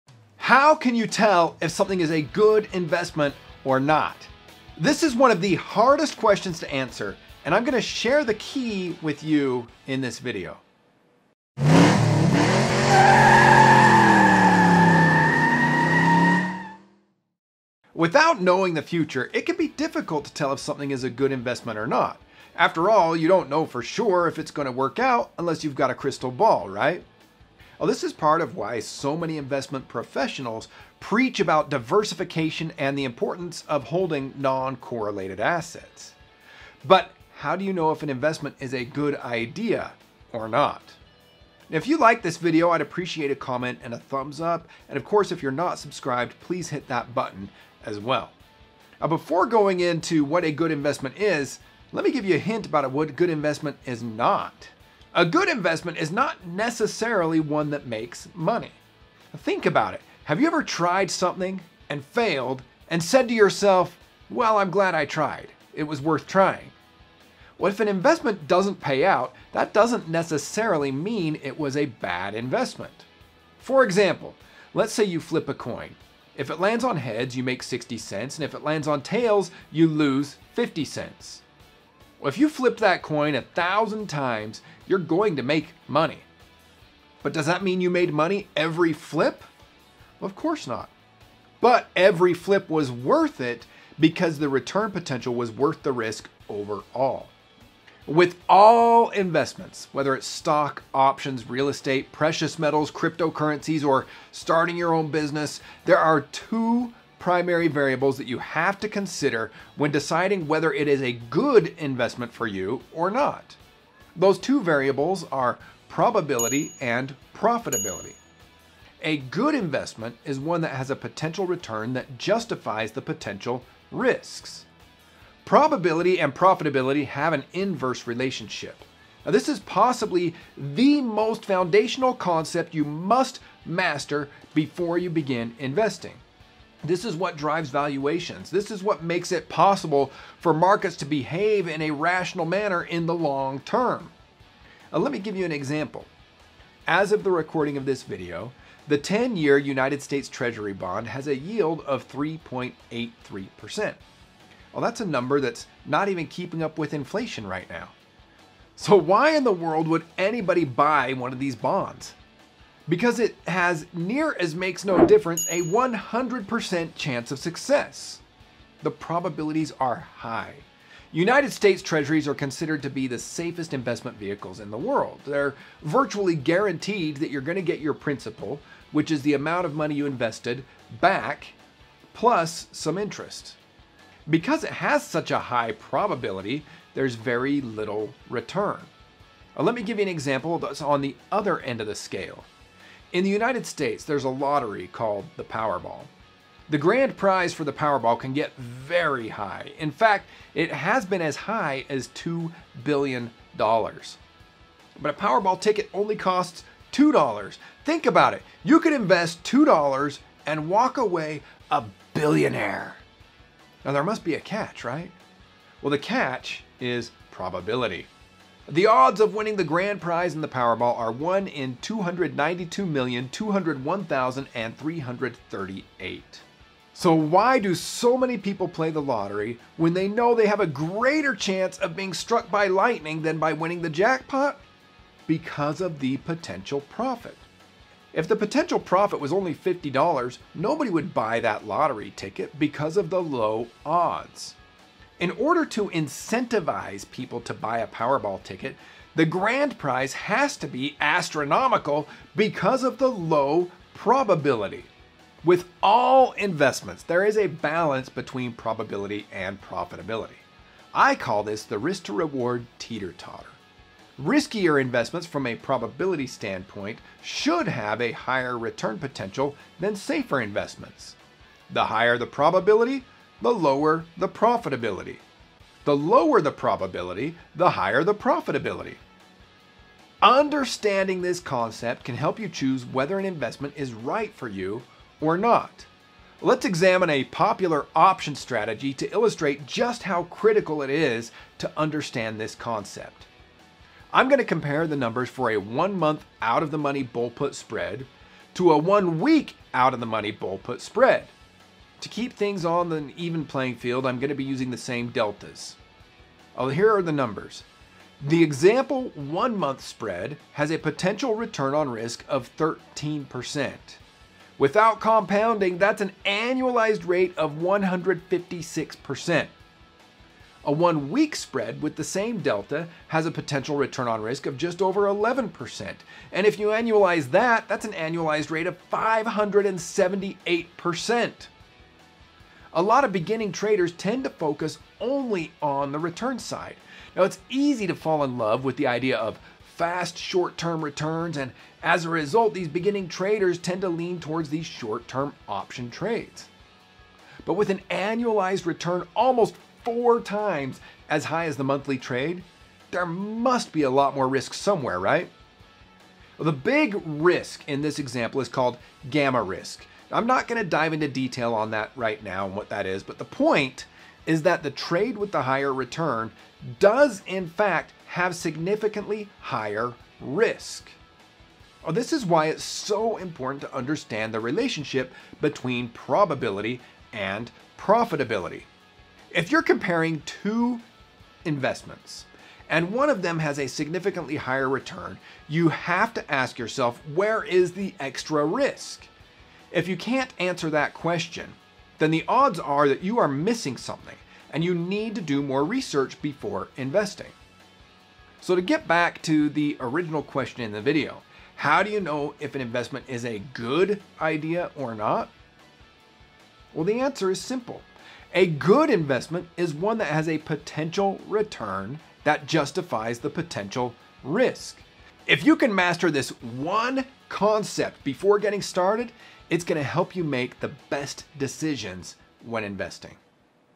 This Simple Concept is Critical to Your Investment Success
Understanding the relationship between probability and profitability is a foundational concept that can help you choose between multiple investments. This is particularly true for option trading as investing using options can give you multiple choices for the same stock forecast. The stock market and option investing can be complex but concepts like understanding probability and profitability can simplify your investment choices.
In this video we compare extreme choices like Treasury Bonds vs Lottery tickets and illustrate how understanding the concepts taught can help you choose between bull put spreads.